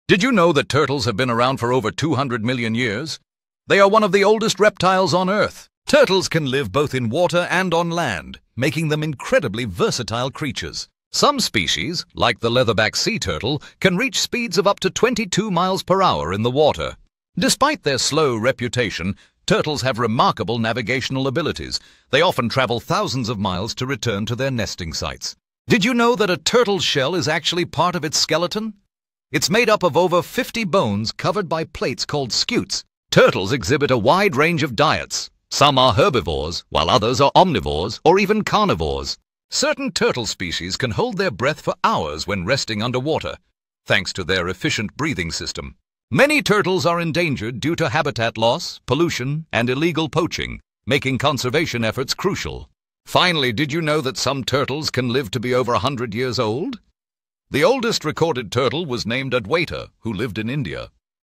Turtle Facts That Will Blow Your Mind:
Dive into the fascinating world of turtles, one of Earth's oldest reptiles, having existed for over 200 million years. Discover their versatility as they thrive in both land and water environments, and learn about the incredible speed of leatherback sea turtles, reaching up to 22 miles per hour. Marvel at their remarkable navigational skills that allow them to journey thousands of miles back to their nesting sites. Uncover the secrets of a turtle's shell, a unique part of its skeleton, and explore their diverse diets that range from herbivorous to carnivorous. As we delve into their amazing adaptations, we also highlight the threats they face and the crucial conservation efforts needed to protect these ancient creatures. Join us in celebrating the longevity of turtles, some living over a century, like the legendary Adwaita.